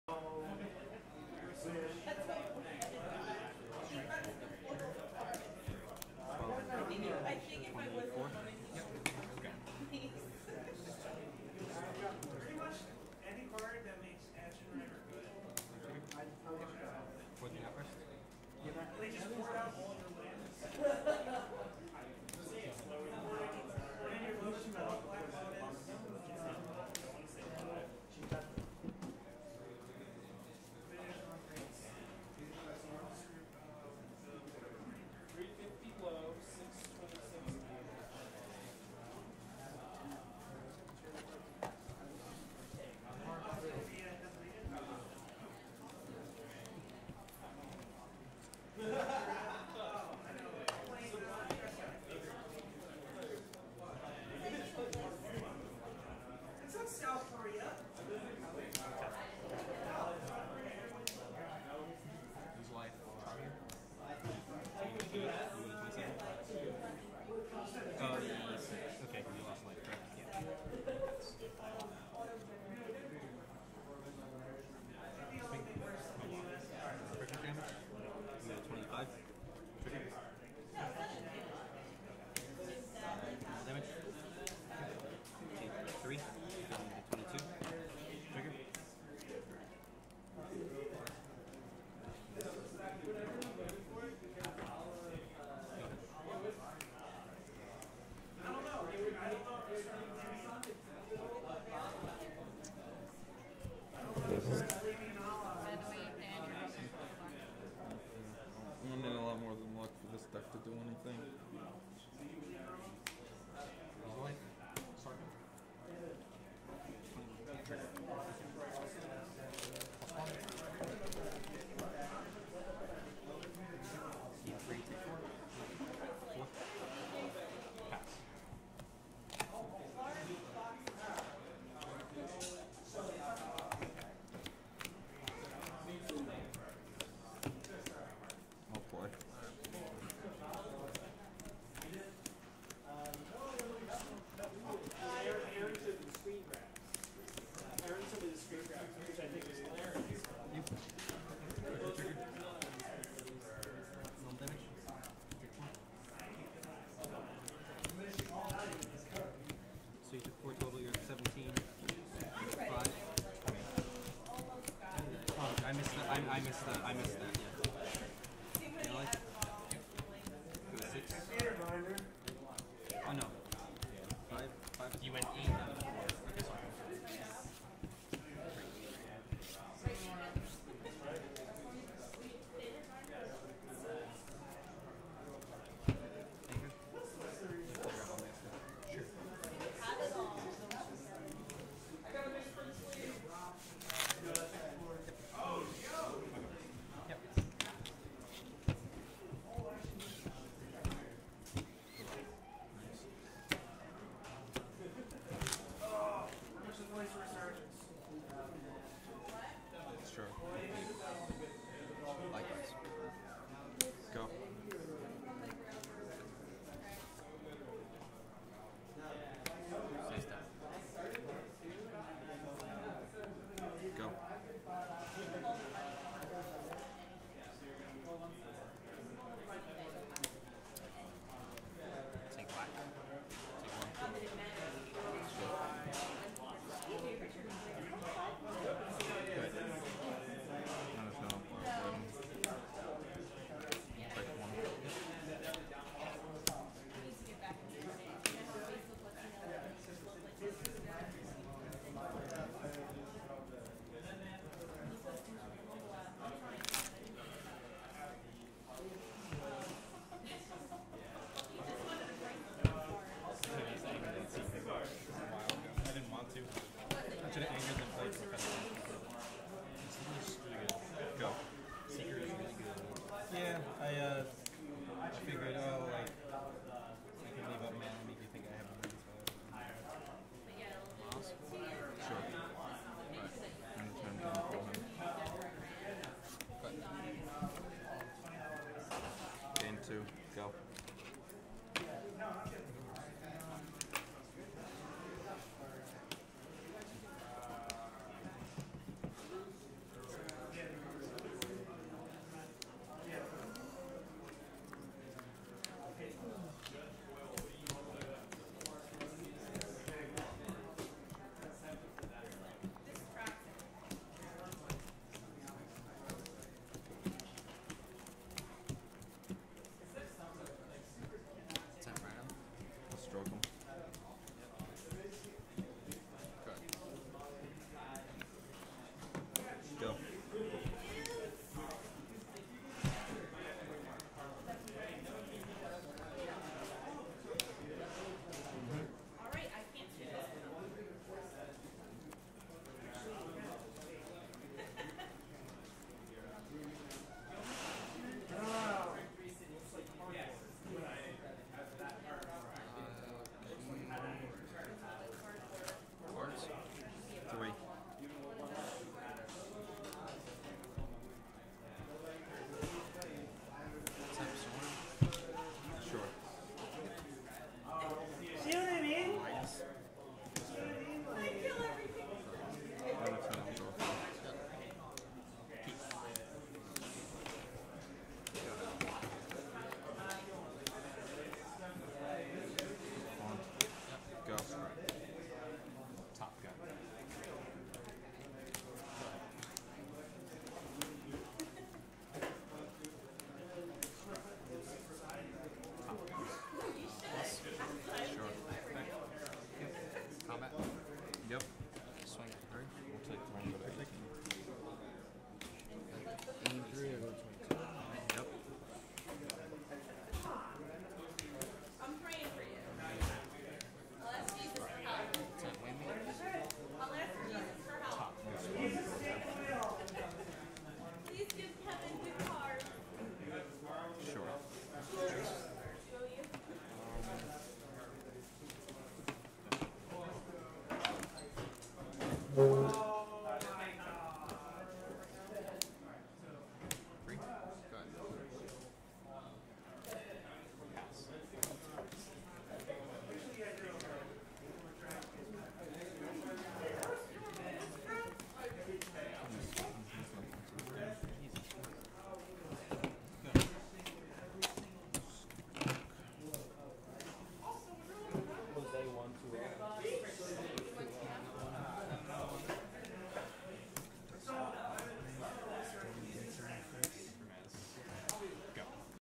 Dan Samoles and Jose Mercado play FNM [Quarterfinals] @ East Coast Gamers
Dan Samoles (Dimir Control) and Jose Mercado (Abzan Midrange) play at East Coast Gamers on November 7, 2014.